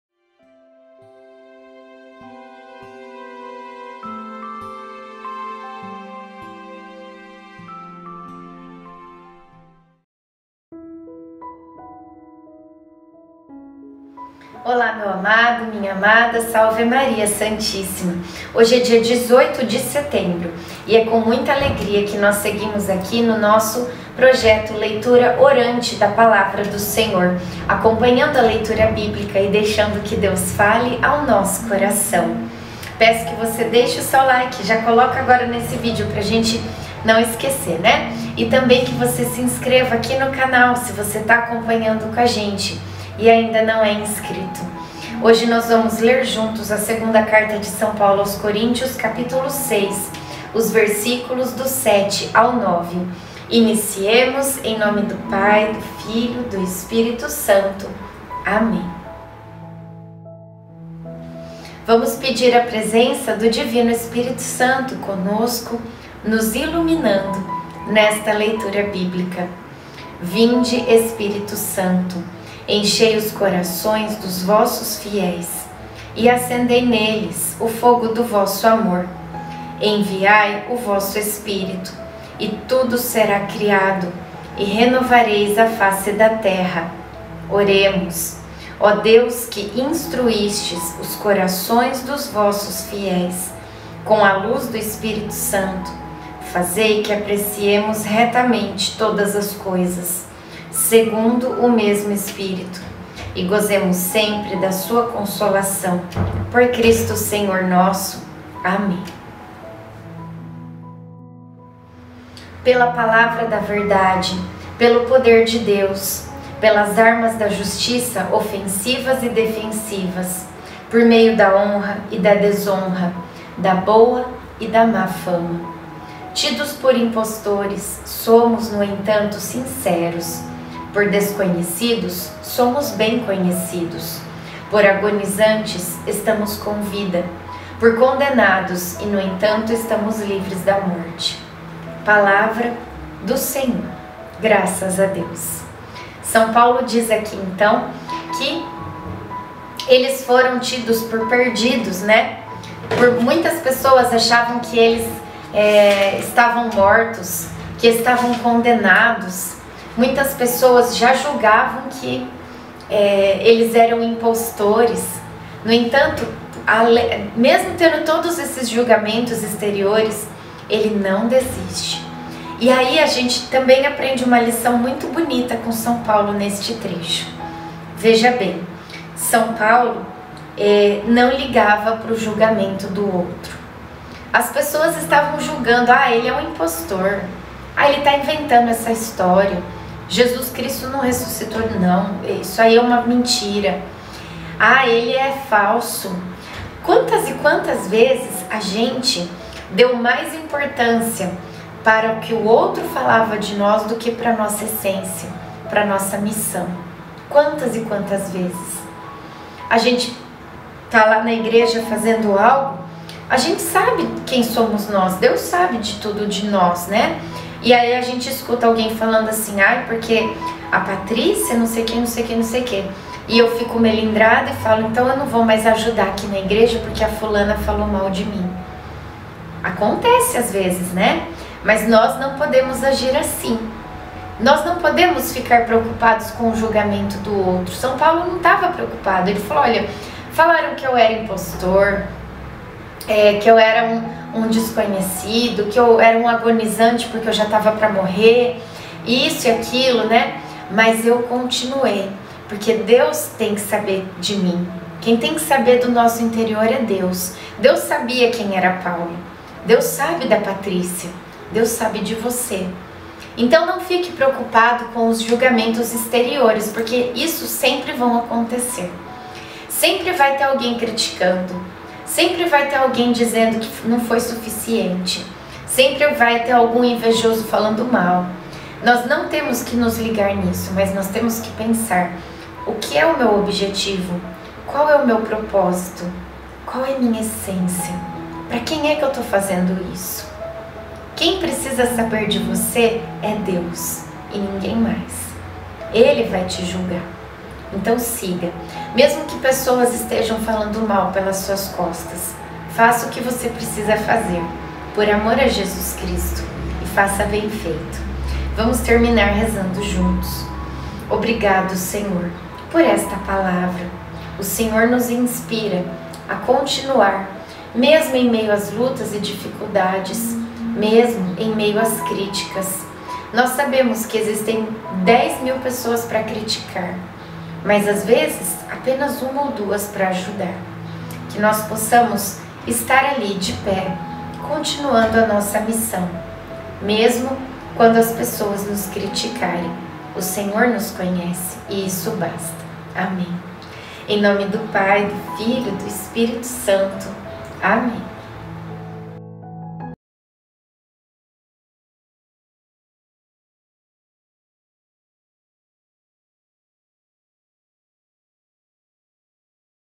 LEITURA ORANTE DA PALAVRA DO SENHOR 18/09/23
II Coríntios - 6

7pela palavra da verdade, pelo poder de Deus; pelas armas da justiça ofensivas e defensivas,* 8por meio da honra e da desonra, da boa e da má fama. 9Tidos por impostores, somos, no entanto, sinceros; por desconhecidos, somos bem conhecidos; por agonizantes, estamos com vida; por condenados e, no entanto, estamos livres da morte.

Deus abençoe!